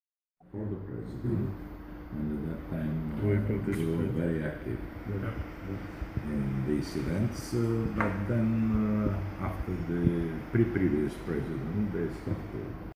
Stati Statev Louis Gerken Dimitar Ivanov
Лу Гъркин, известен американски инвестиционен банкер бе в УНСС и се среща с проф. Стати Статев, ректор.